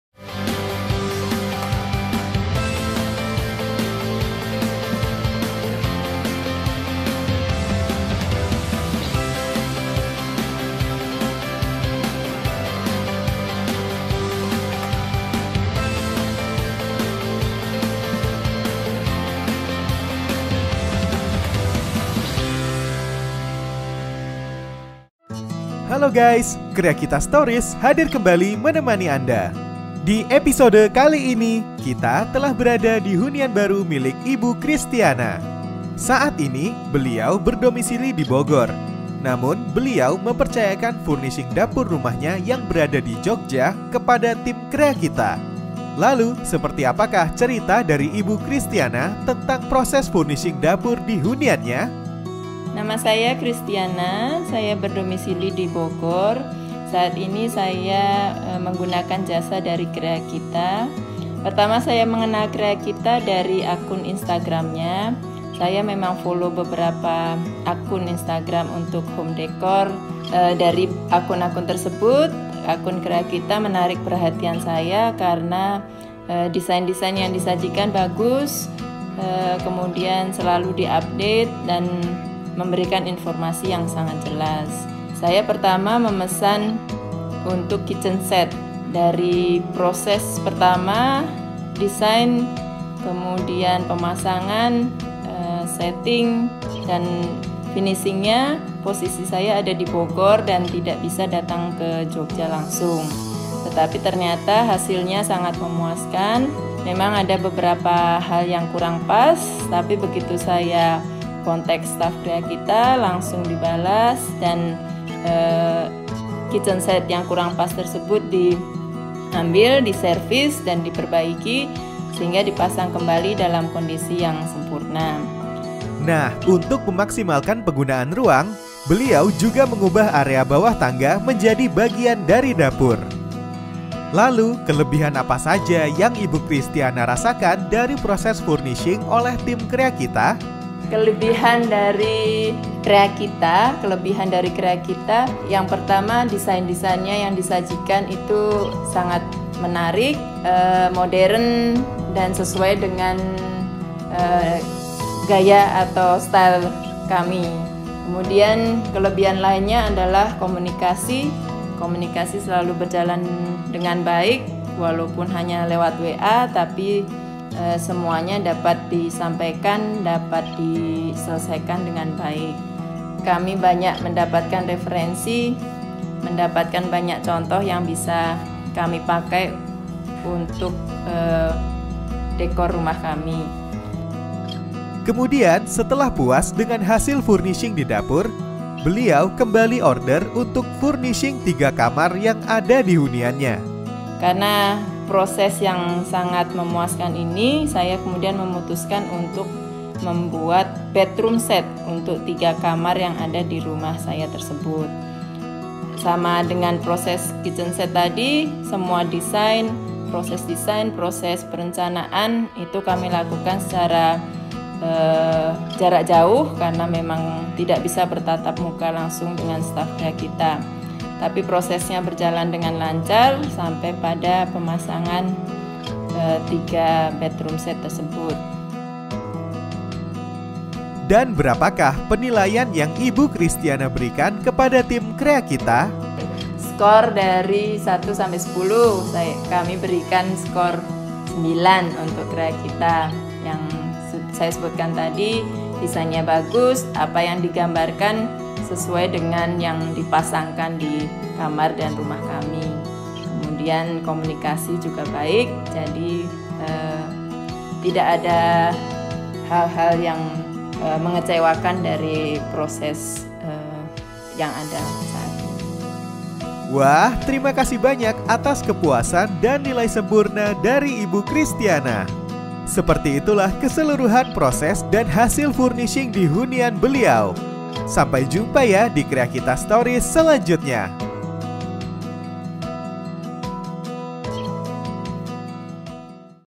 Desain & Bangun Interior Rumah Singgah || Kitchen Set Bawah Tangga || Bedroom Set
Ibu Christiana mempercayakan Kreakita untuk mendesain dan membangun interior untuk rumah singgahnya di Yogyakarta. Mulai dari dapur di bawah tangga dan bedroom set untuk 3 kamar tidur dirumahnya.

untuk kitchen set menggunakan material multiplex lapis HPL. Sedangkan kamar tidur menggunakan material full kayu pinus solid dengan finishing kombinasi natural dan duco hitam.